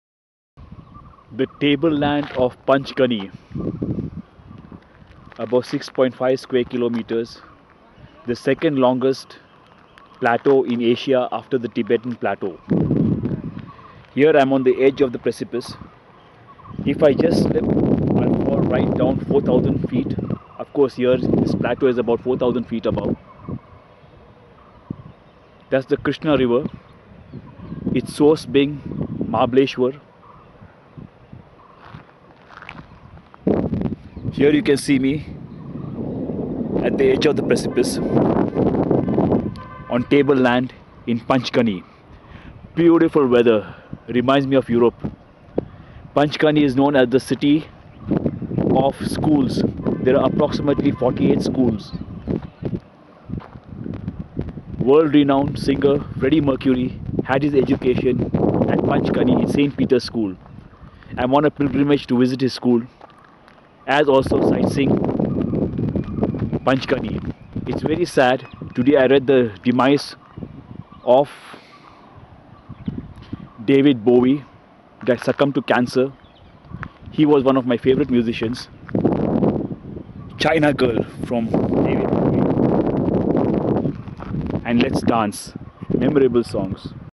A glimpse on the edge of TABLELAND in Panchgani
huge barren land resembling a playing field with a steep drop into the valley below. Tableland is 4550 feet above sea level and the highest point in Panchgani as also the second highest plateau in Asia after the Tibetan plateau..The plateau has a area of 6.5 Kms and i seemed to be the first Mumbai tourist trekker to have set foot on Tableland on Tuesday(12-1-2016).Read my blog :- Seafarer/Blogger/Traveller Rudolph.A.Furtado